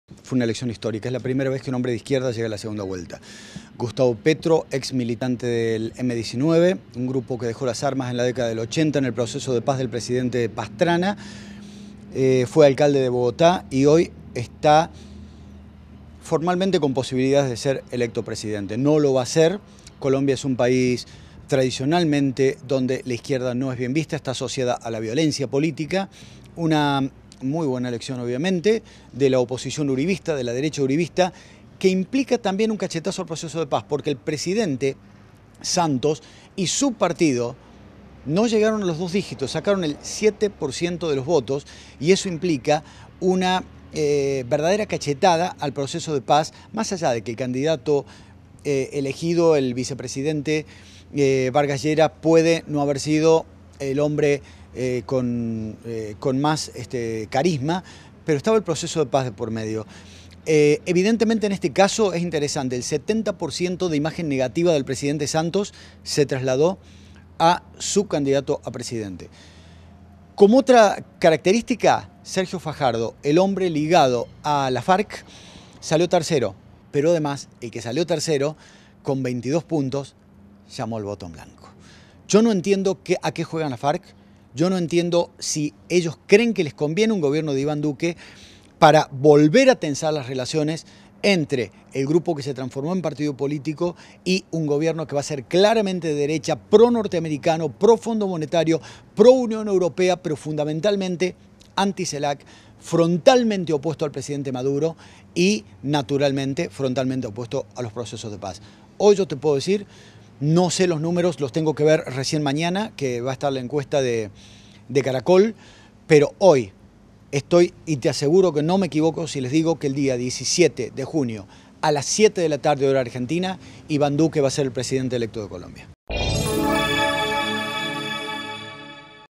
ELECCIONES PRESIDENCIALES EN COLOMBIA - PABLO WEHBE
EL 17 DE JUNIO SERÁ LA SEGUNDA VUELTA ENTRE IVÁN DUQUE Y GUSTAVO PETRO
Facebook.com/uniriotv
Twitter.com/uniriotv